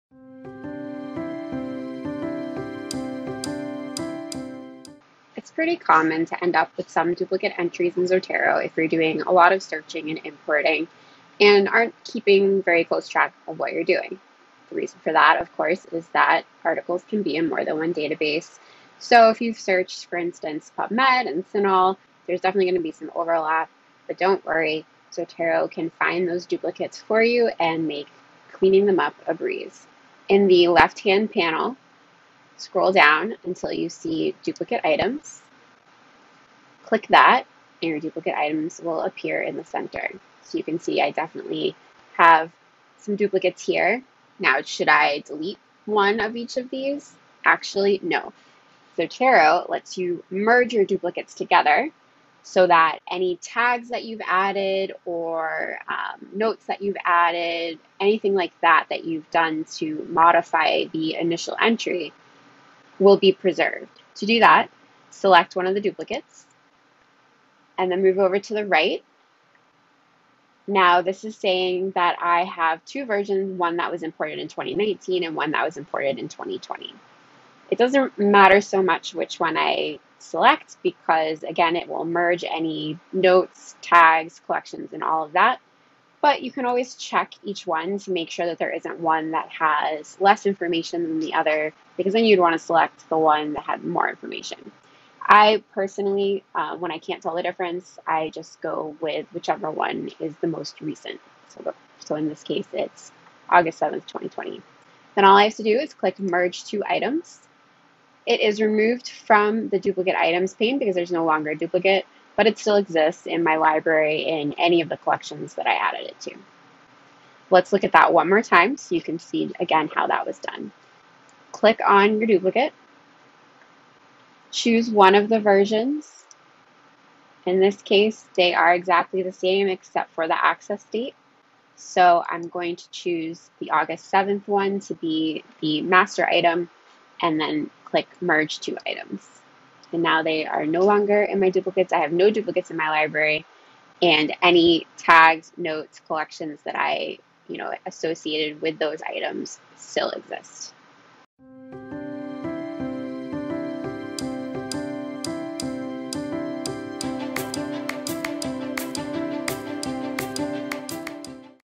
Identifying and Merging Duplicates in Zotero
How to identify and merge duplicate items in Zotero. From the Bellack Library's Zotero Workshop.